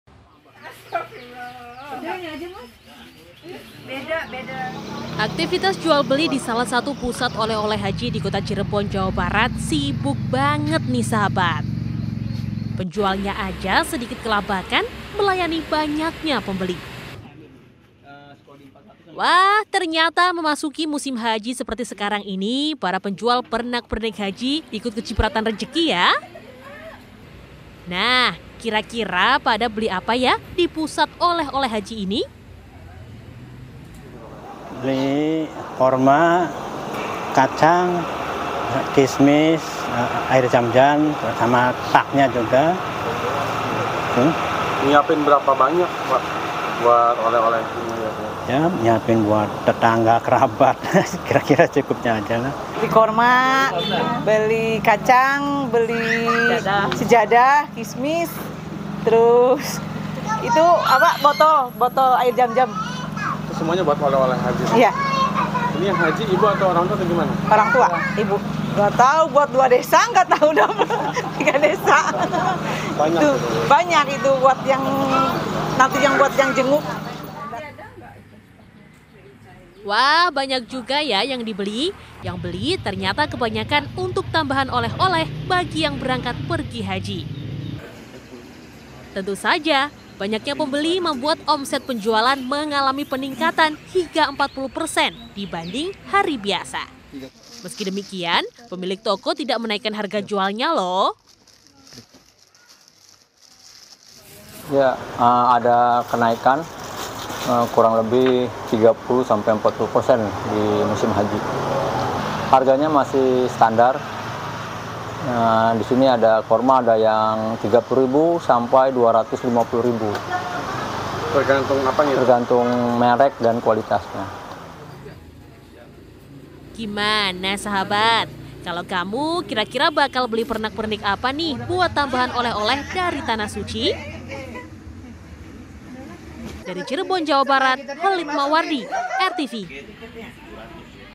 Musim Haji, Pusat Oleh-oleh Haji Laris Manis
Musim haji ternyata berdampak manis pada penjual oleh-oleh haji. Meski calon jemaah haji baru sebagian yang berangkat ke tanah suci, sejumlah pusat oleh-oleh haji sudah diserbu oleh para calon jemaah haji. Omset penjualan pun meningkat hingga 40 persen dibandingkan hari biasanya.